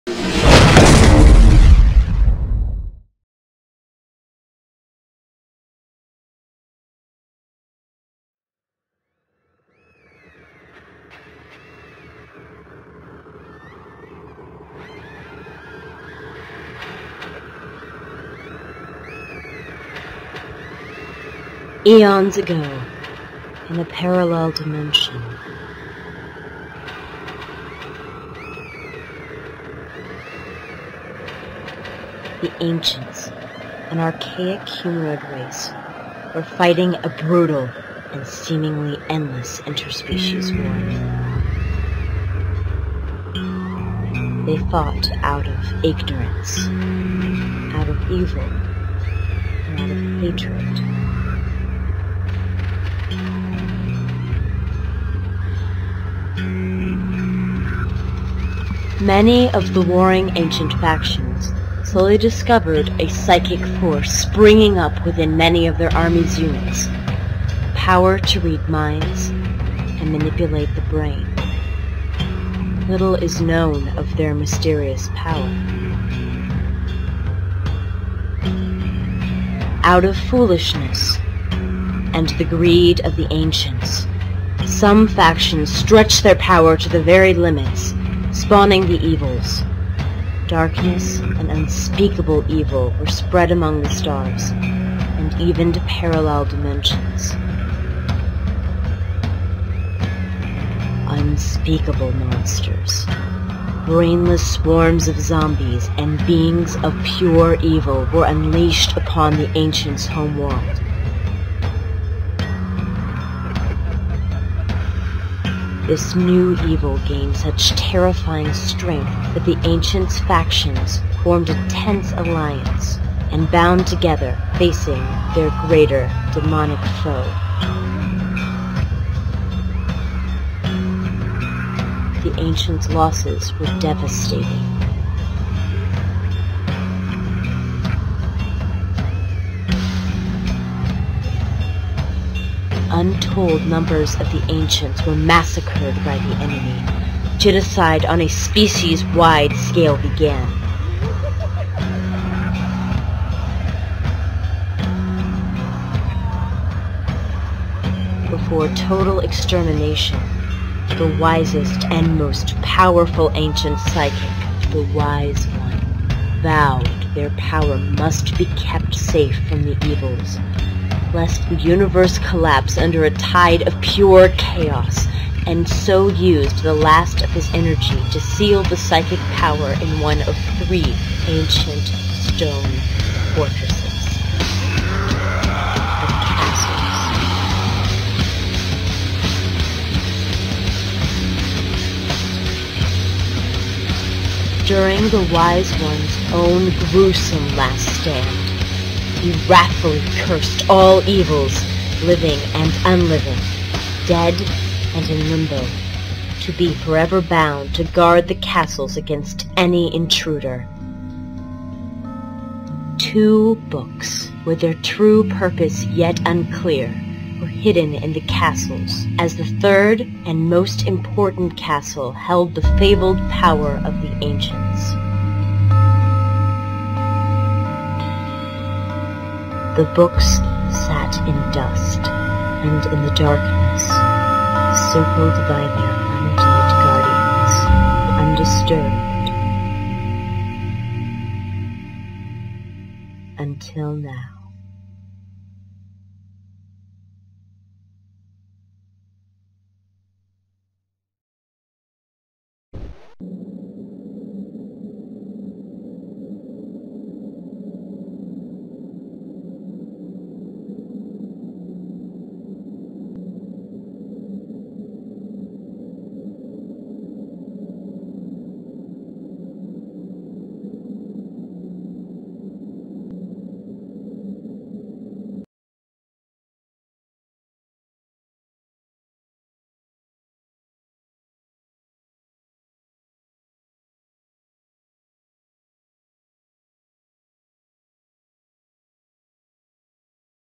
Castle Series - Castle Prologue (1080p)
Official upload in 1080p!

The prologue sets the background story and is meant to be watched before starting Castle III.

For the ultimate source of Castle Series information and updates, find me and the series' progress

All music is properly listed in the credits, and belongs to the owners.